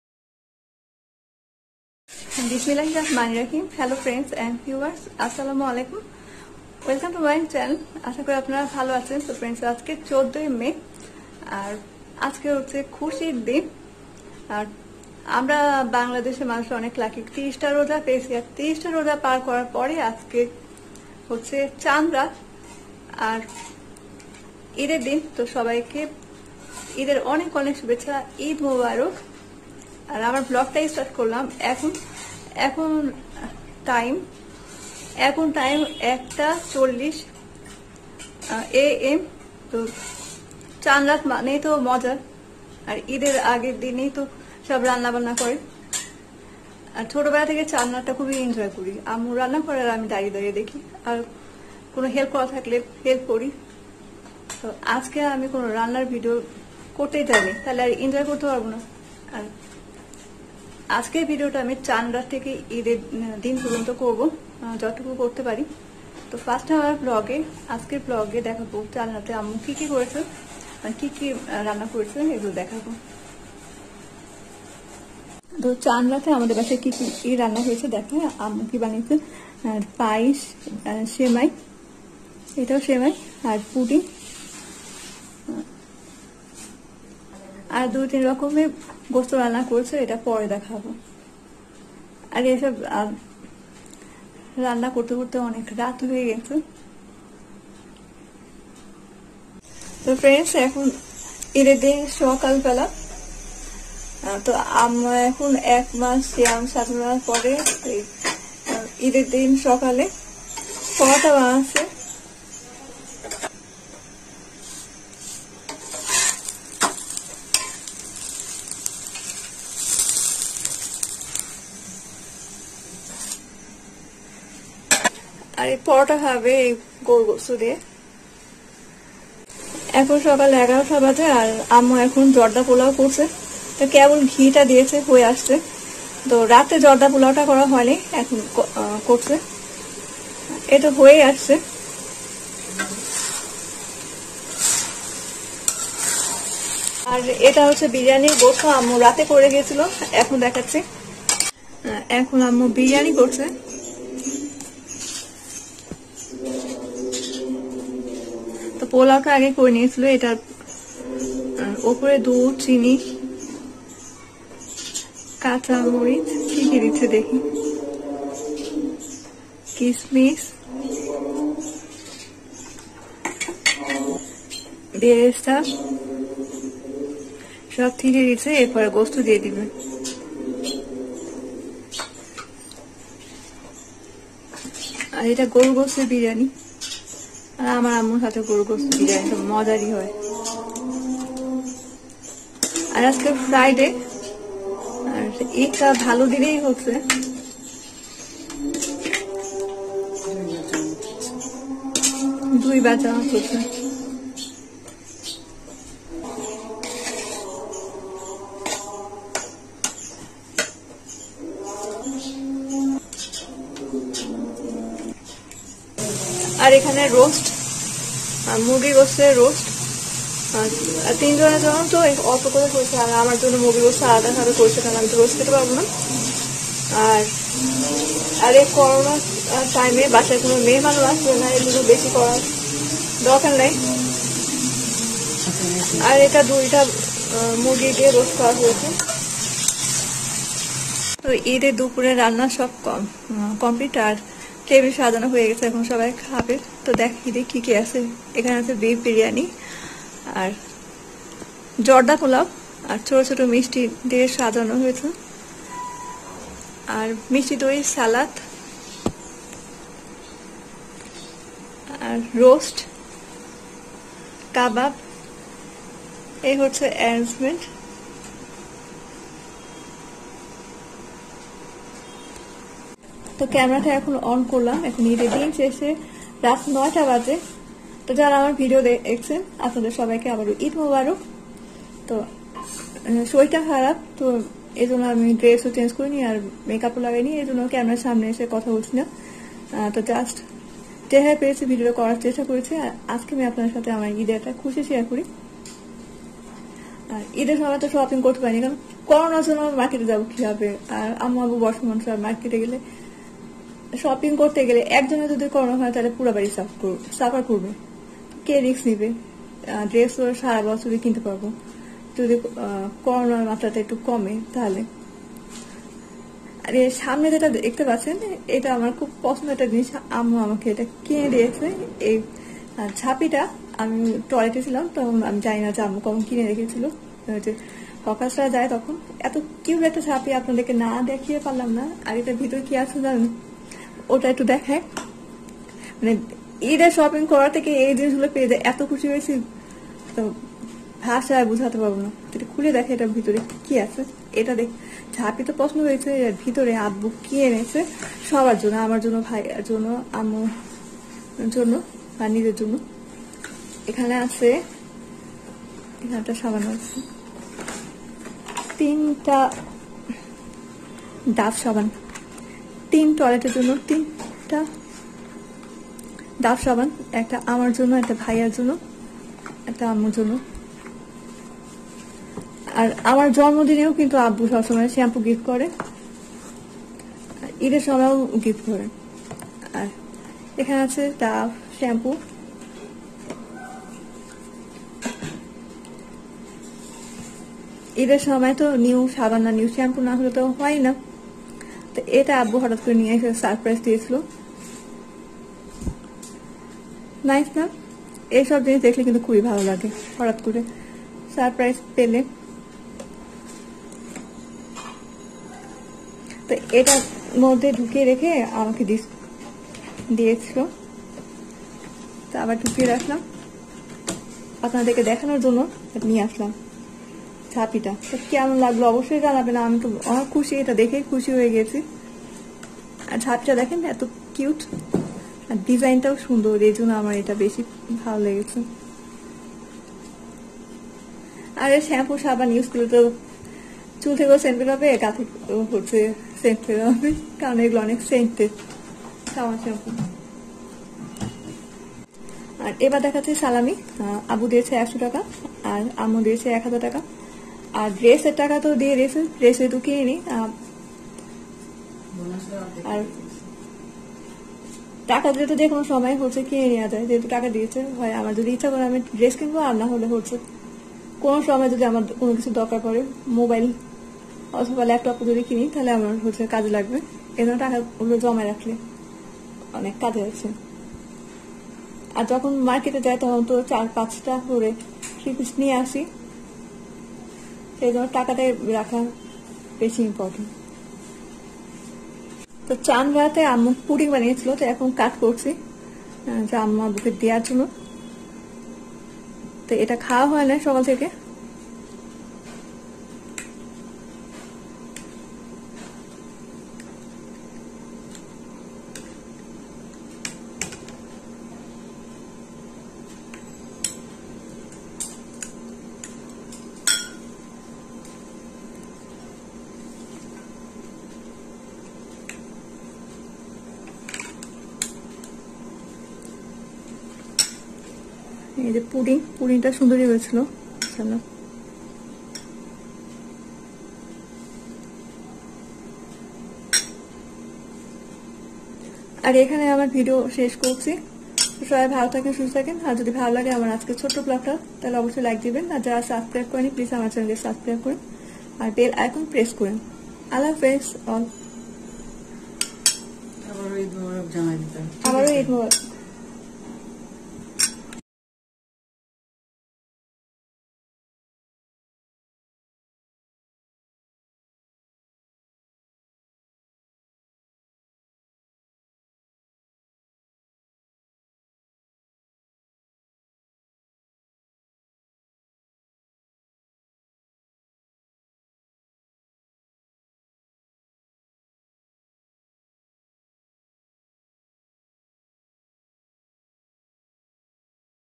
Chand Rat to Eid Special Vlog2021.Ammu &Abbu Eid salami koto Tk dilo&Ammu ki surprise dilo
Chand Rat to Eid Special Vlog 2021.Ammu &Abbu Eid salami koto Tk dilo&Ammu ki surprise dilo